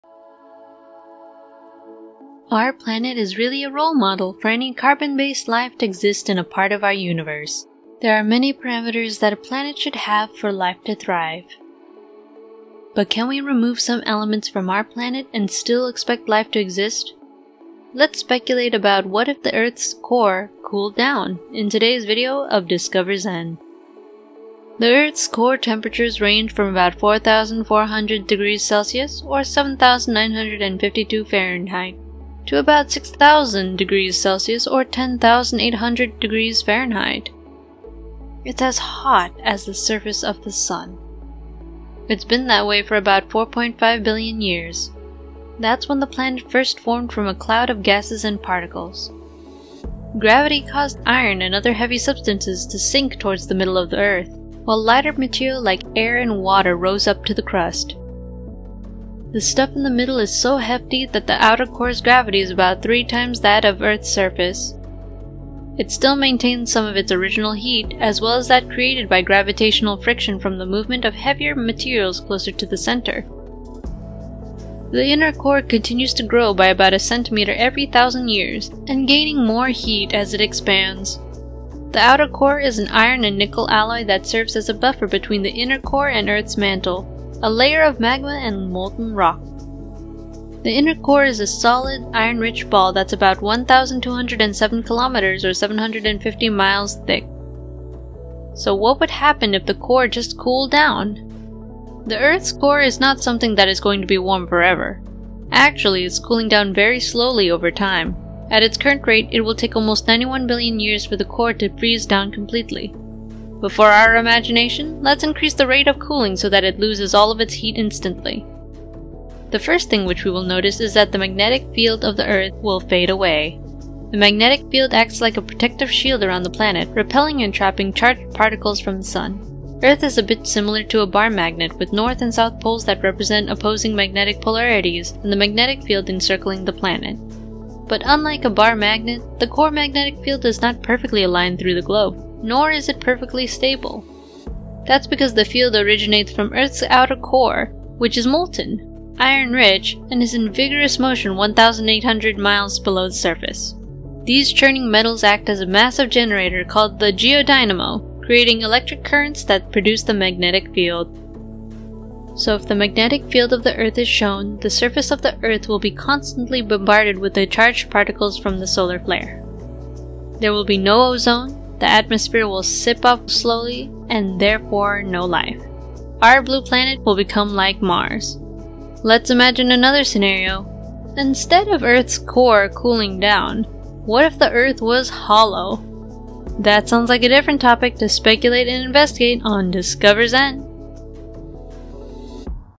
What If The Earth's Core Cooled Down?
There are many elements in our Solar System that made life possible on Earth. One of those is the Earth's Core. But What if the Core was different? Let's Speculate about "What If The Earth's Core Cooled Down?" in today's video of DiscoverZen.

Enable the notification icon if you want to be notified when we upload our next video.

Credits:
Video Script, Research, and Marketing by K.P.Sajith and Sarthak Kumar
Video Production by Rahul B, James, and Maria
Voiceover by Voice actress Sarante